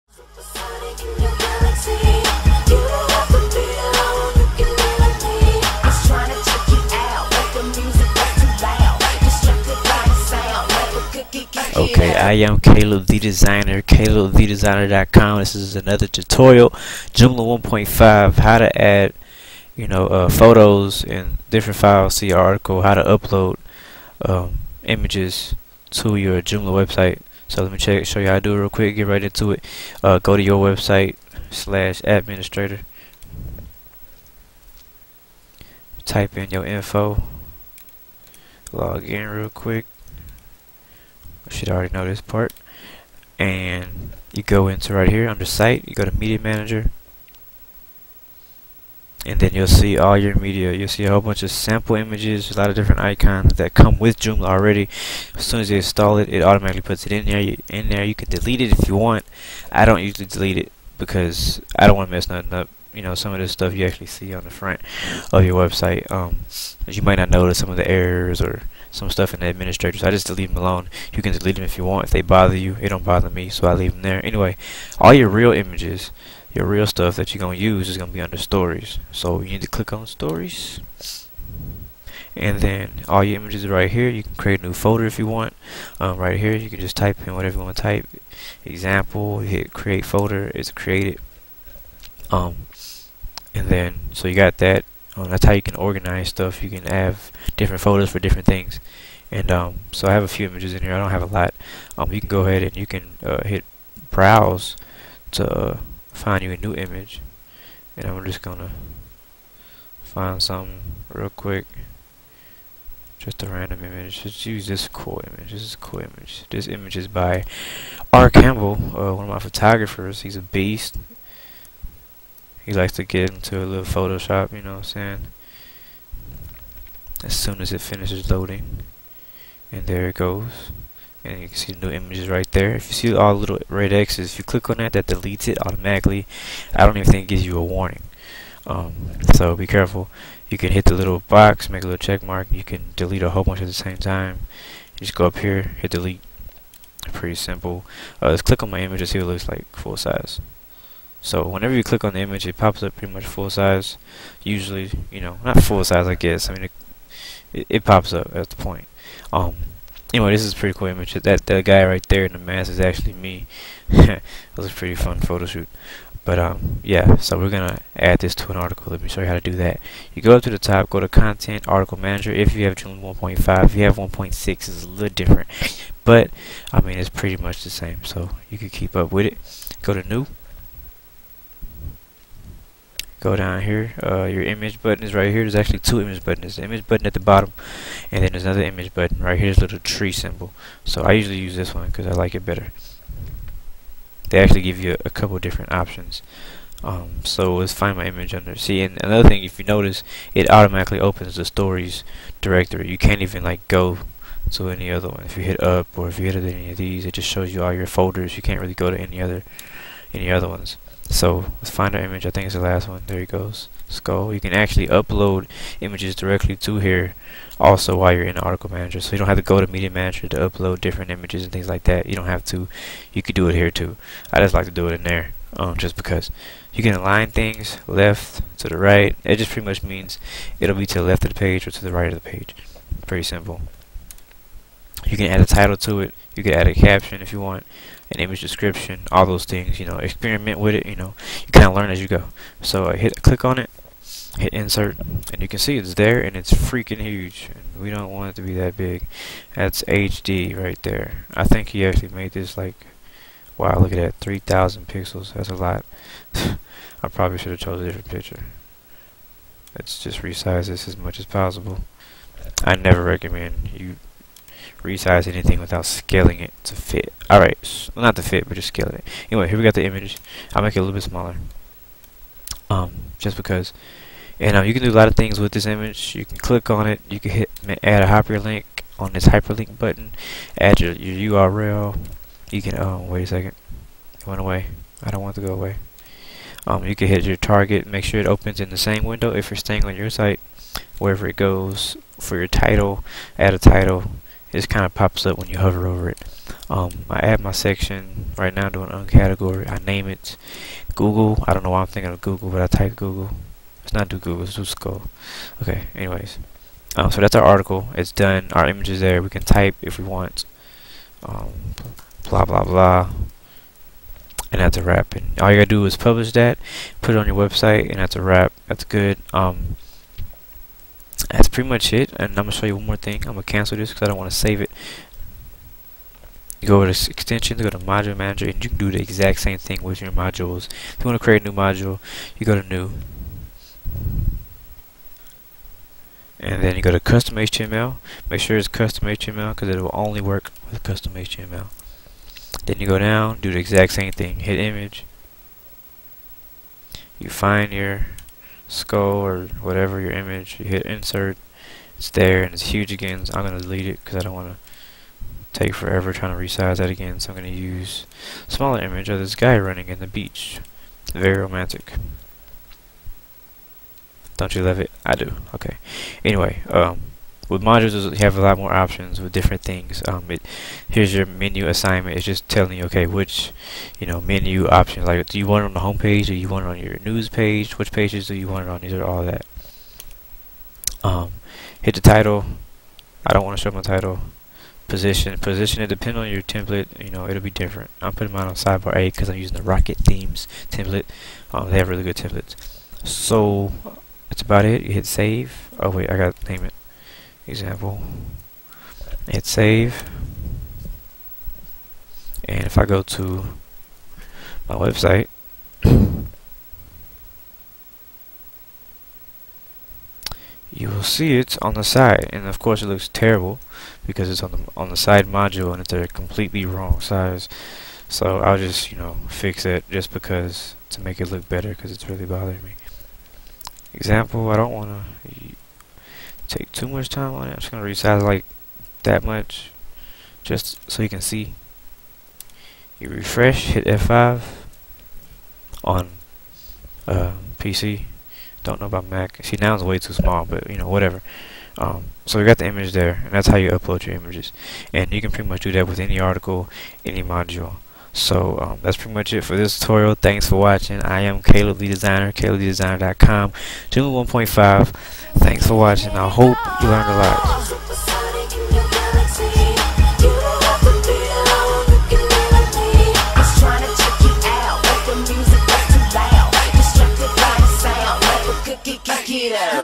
Joomla - Media Manager Tutorial
Joomla 1.5 Media Manager Tutorial
How to upload images & other files to your Joomla CMS website